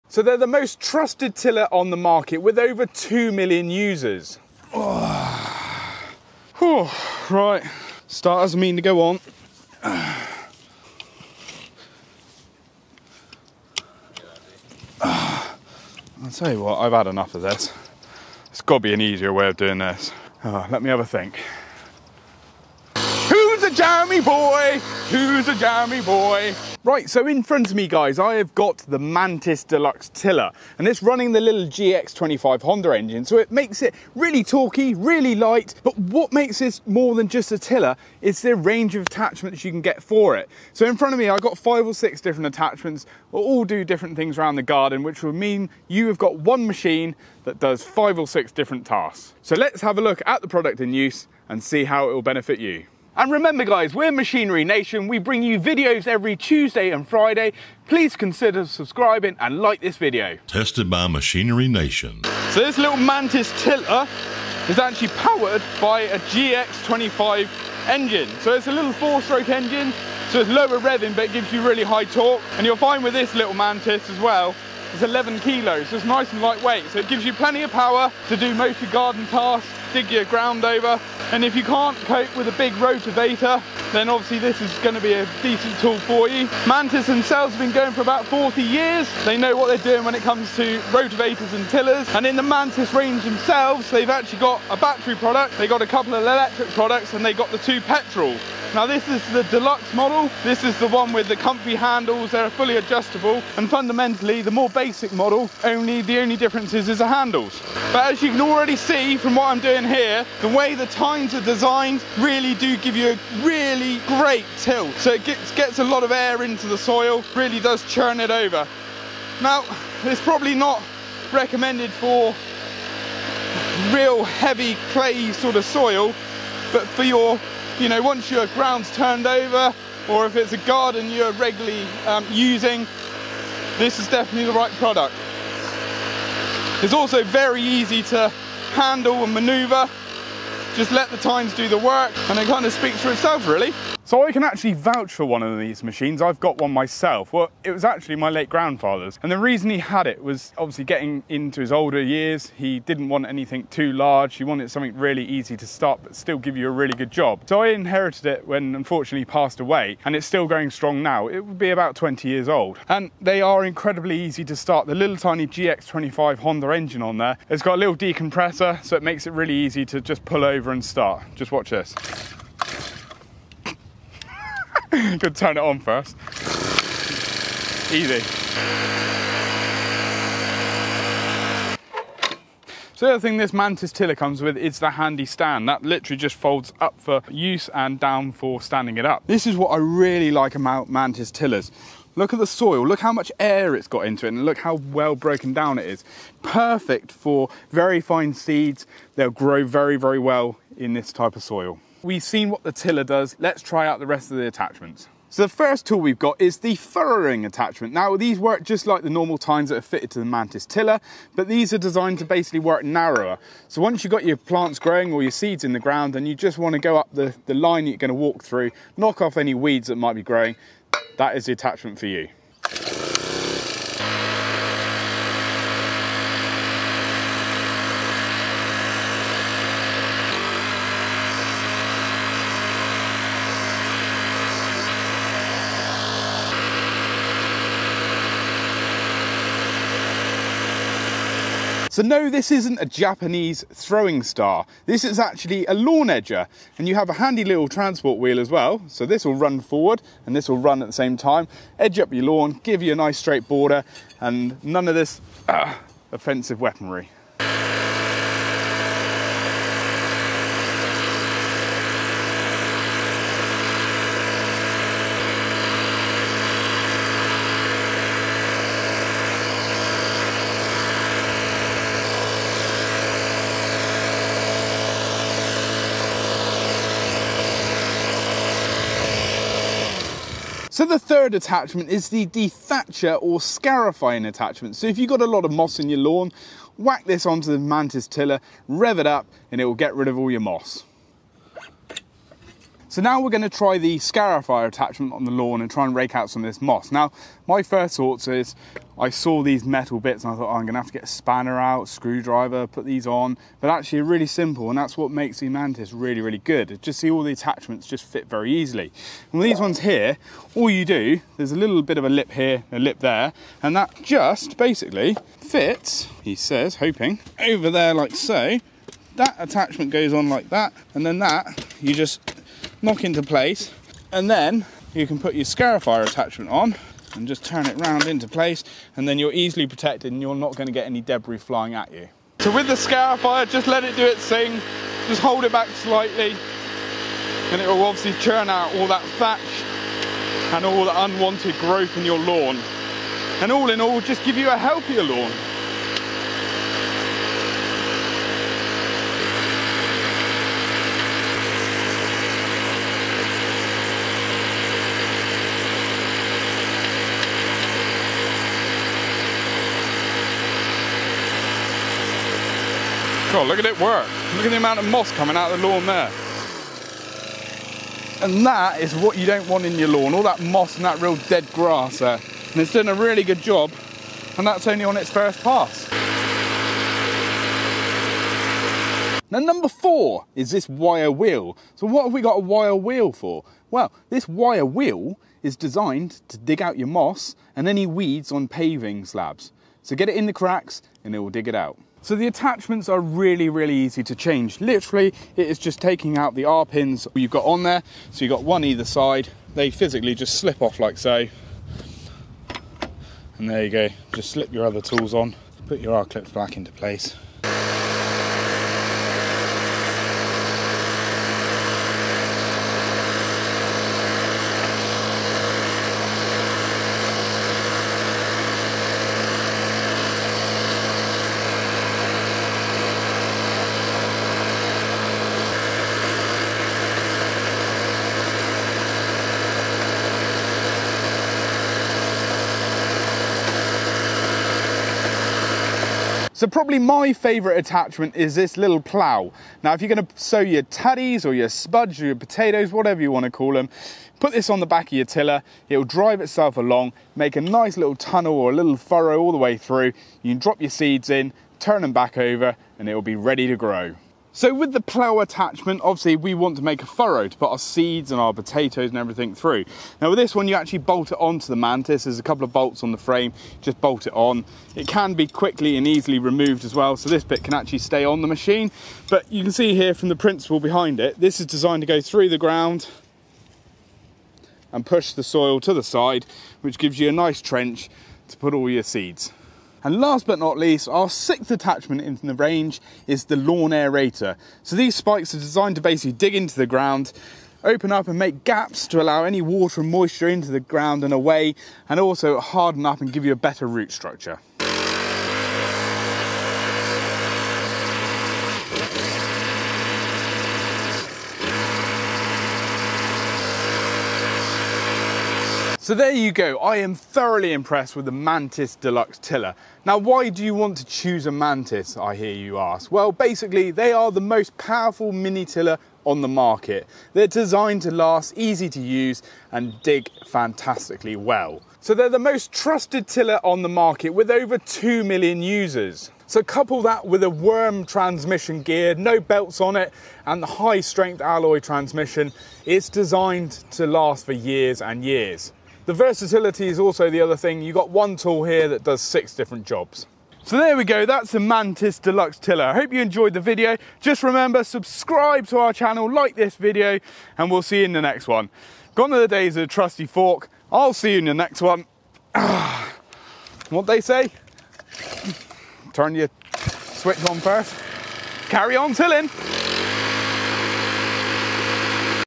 Is The MANTIS Mini Tiller Just A Mini Tiller?
Josh checks out the Mantis Mini Tiller and all of the different attachments that you can buy to add to your machine. Scarifier - Aerator - Edger - Narrow Tines - Honda Engine - Easy Start - Mini Rotavator - Cultivator - Garden Plough

Is The MANTIS Mini Tiller Just a Mini Tiller?! What else can you do with this machine?

Aspen Fuel is a leading UK distributor of Aspen Products including Aspen Alkylate Petrol, Aspen Alkylate Fuel and Aspen Oil.